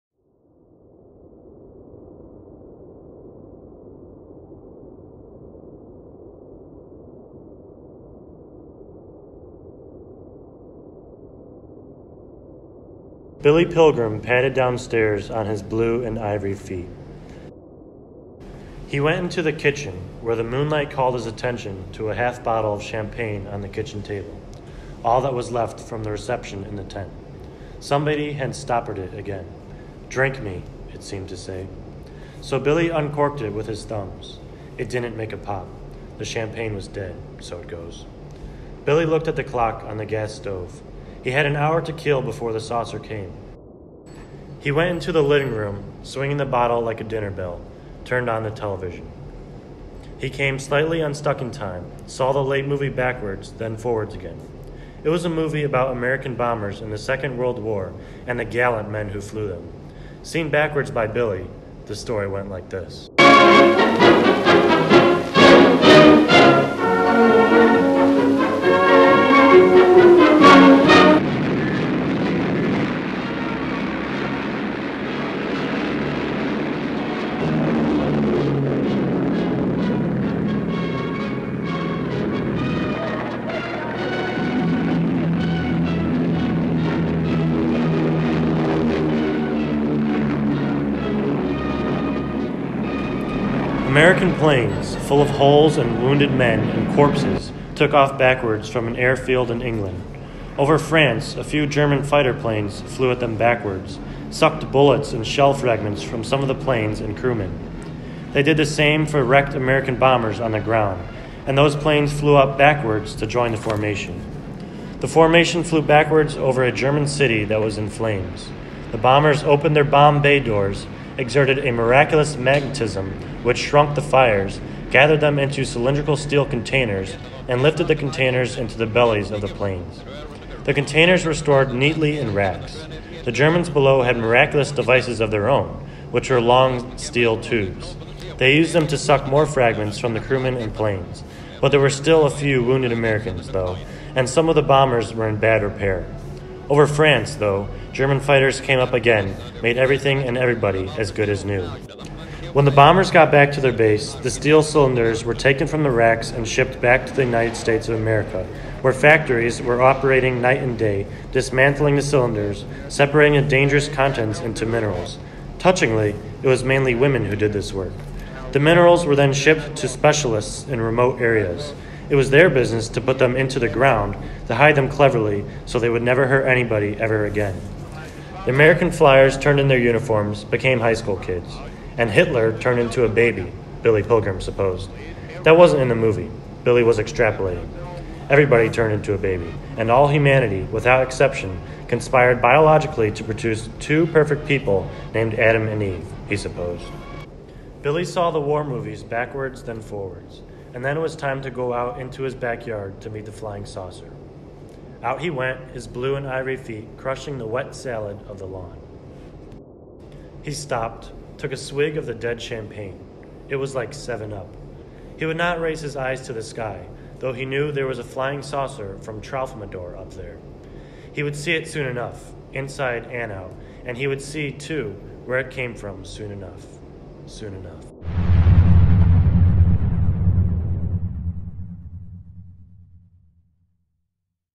Slaughterhouse V
A scene from Slaughterhouse V filmed for a school project.